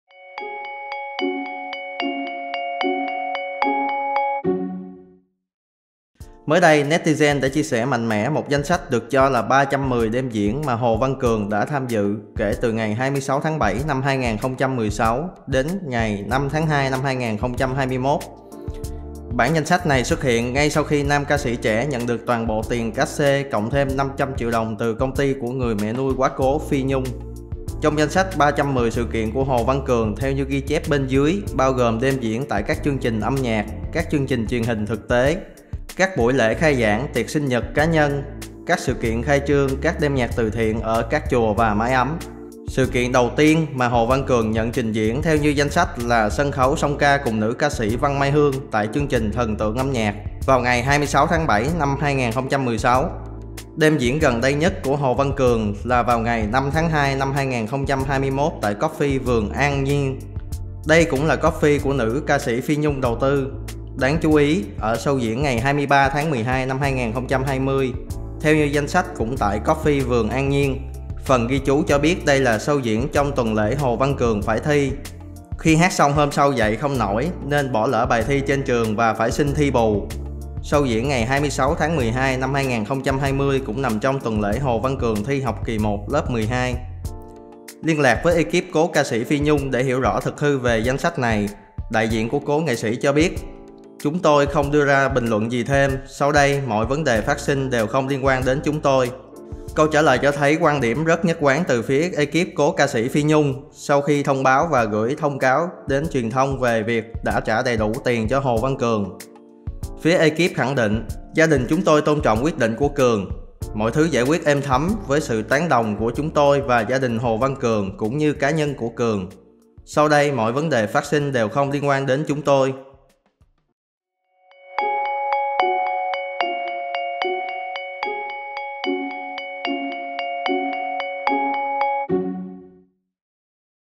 Danh sách 310 đêm diễn và sự kiện của Hồ Văn Cường - Ekip Phi Nhung nói gì ?
. Tối 11/10, ngay sau khi ồn ào về cát-xê của Hồ Văn Cường tiếp tục bùng nổ, netizen đã chia sẻ mạnh mẽ 1 danh sách được cho là 310 sự kiện mà Quán quân Vietnam Idol Kids đã tham dự suốt 5 năm qua kể từ khi đăng quang.